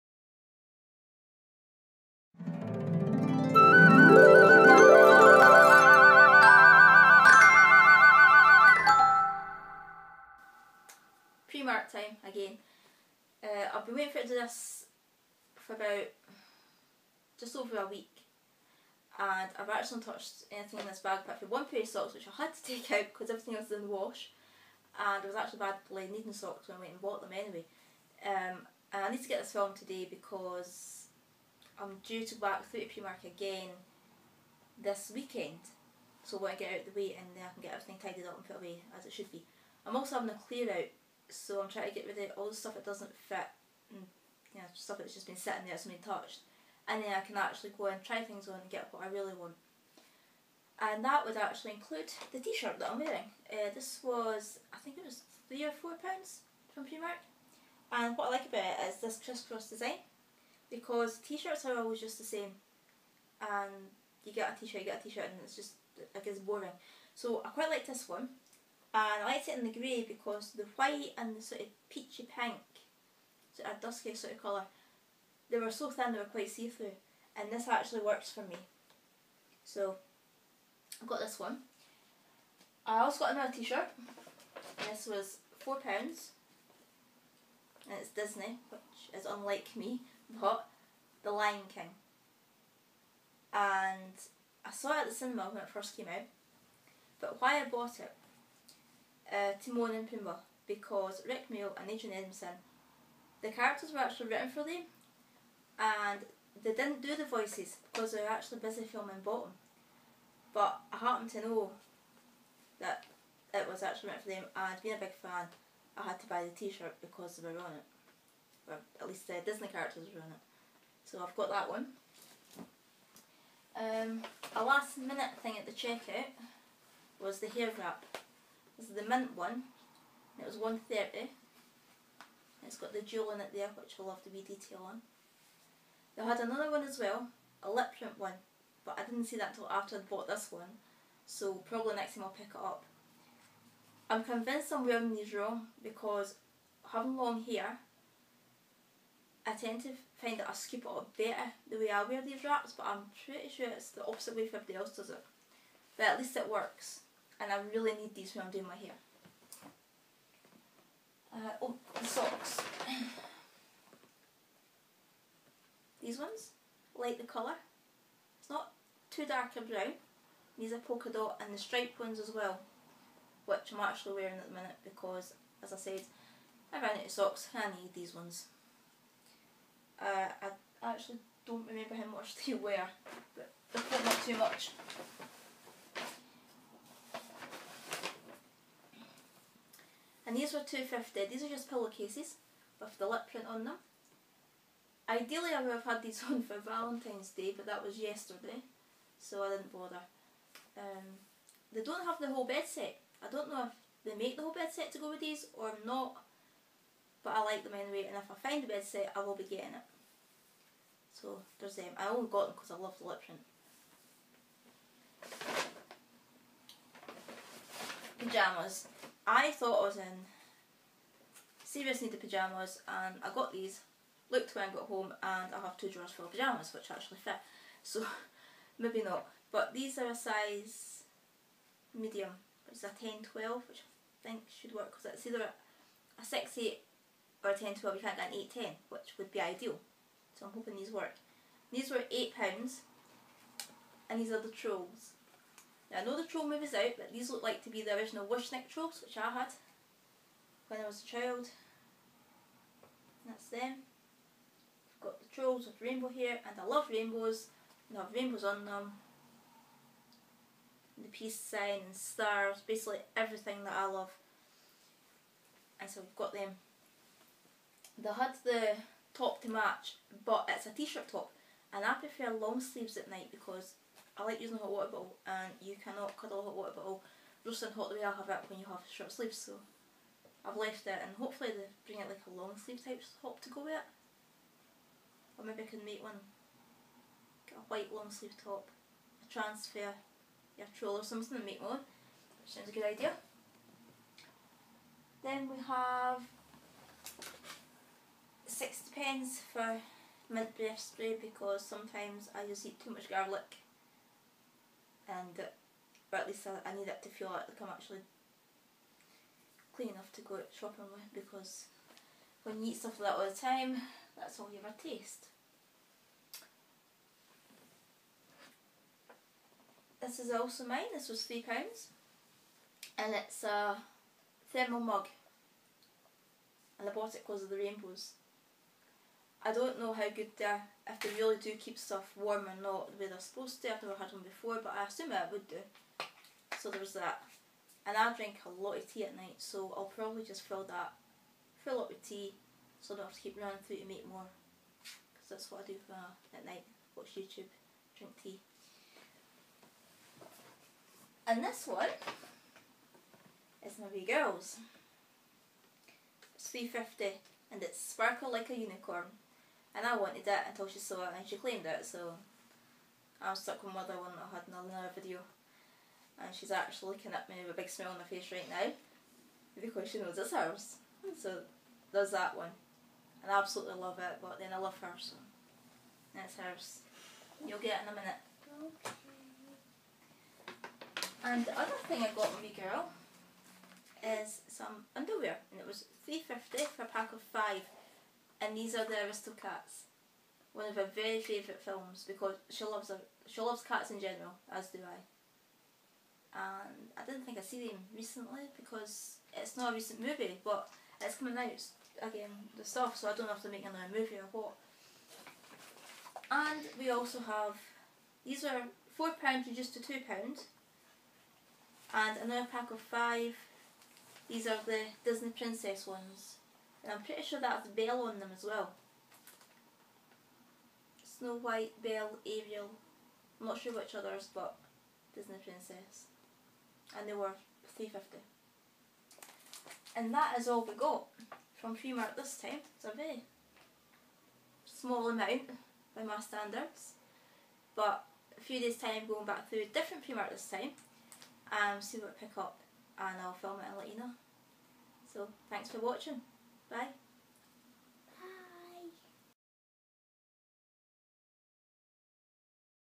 Mini Primark Haul Feb 2017
A mini haul of items bought at Primark in Edinburgh with my daughter. Includes some small Disney kids items.